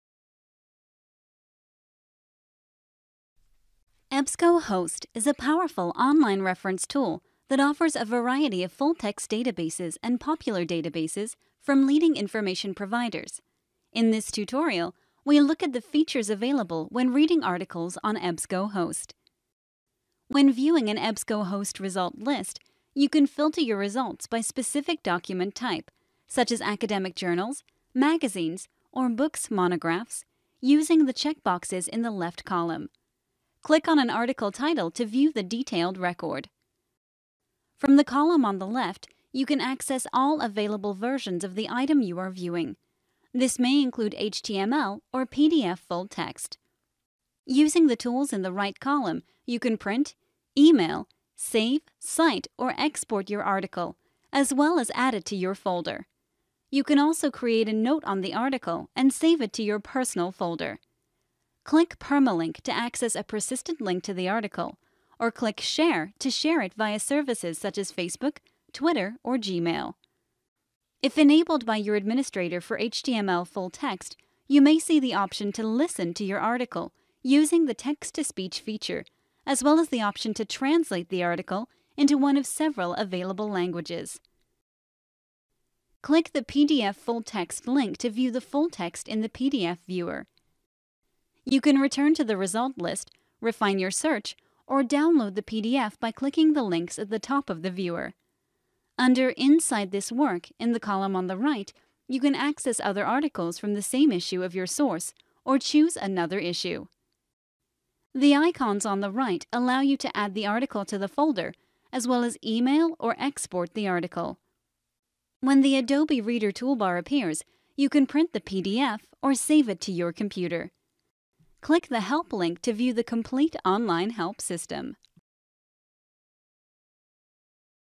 Reading an Article on EBSCOhost - Tutorial
This tutorial demonstrates the EBSCOhost Article Detailed Record as well as features of HTML Full Text and the PDF Full Text Viewer.

and sign in with credentials provided by your library/institution.